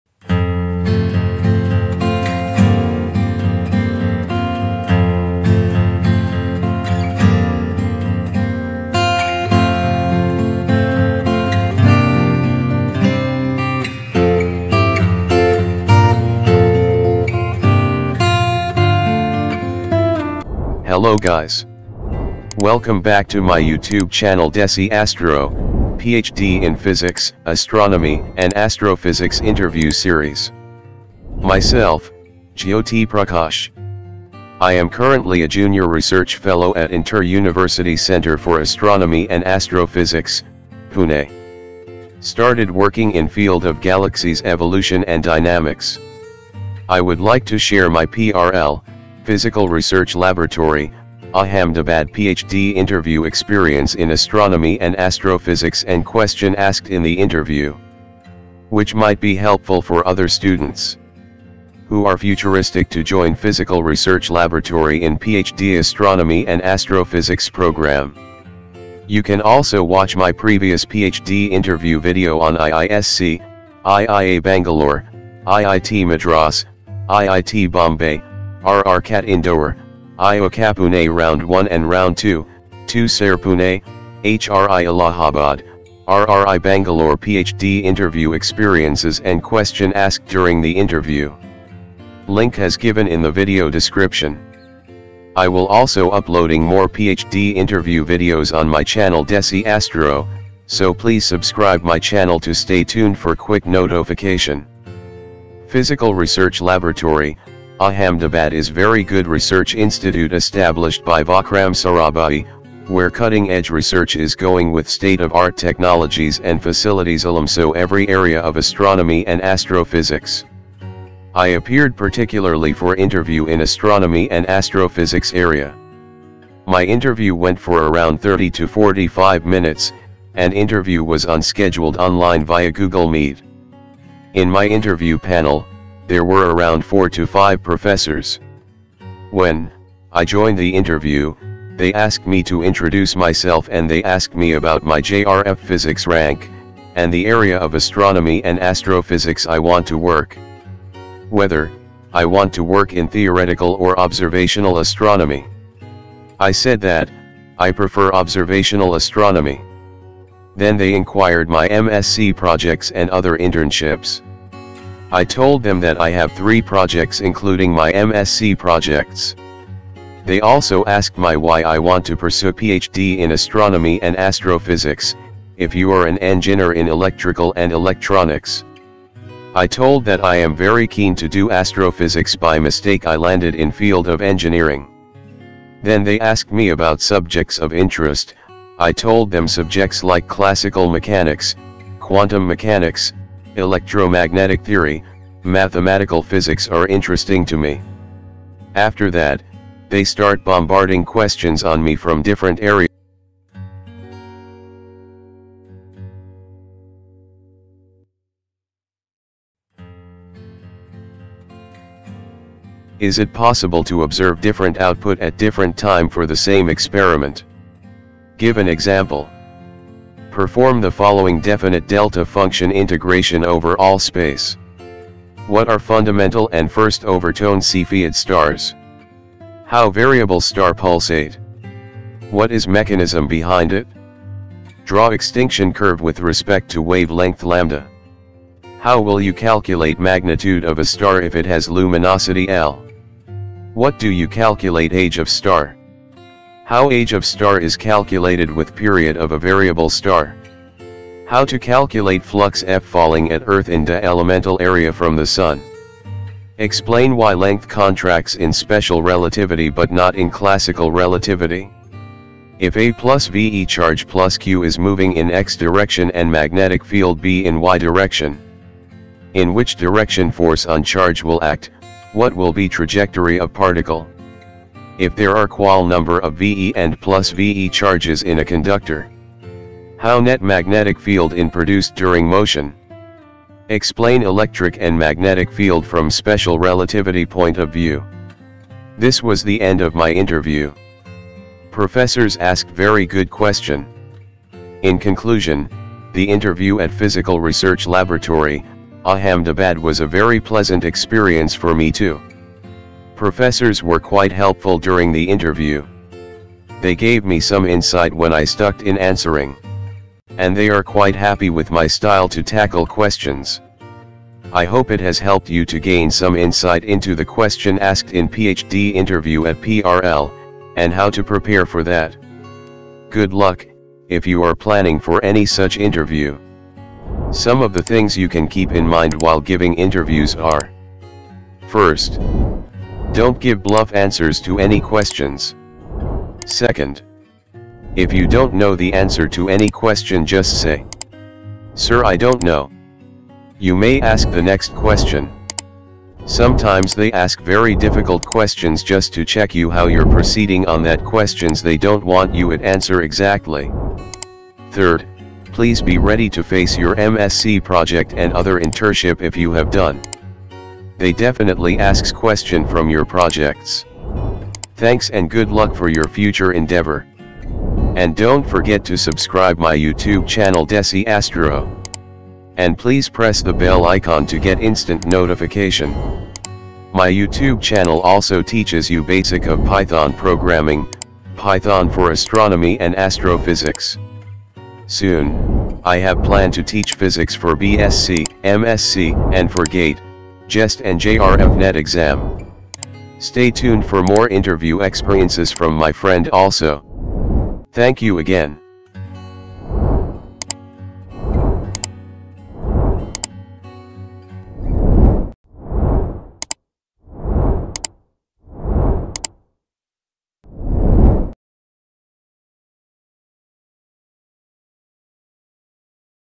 PRL Ph.D. INTERVIEW| Ph.D. Physics Interview Experience - PRL Ahmedabad| PHD ADMISSION
Ph.D. Physics Interview Experience - PRL Ahmedabad, Astronomy and Astrophysics| PRL Ph.D. ADMISSION

In this video, I share my personal experience of interviewing at PRL Ahmedabad, one of the leading research institutes in India. I discuss the interview process, the questions I was asked, and my overall impression of the experience.

During my interview, I was asked a range of technical and behavioral questions, including my understanding of fundamental concepts in my field, my experience with specific research techniques, and my ability to work in a team environment. I was also asked about my motivation for pursuing a career in research and my long-term career goals.

Overall, I found the interview to be challenging but rewarding. The interviewers were knowledgeable and professional, and the questions they asked allowed me to showcase my skills and experience.

If you are preparing for an interview at PRL Ahmedabad or any other research institute, I hope that my experience can provide some insight and guidance.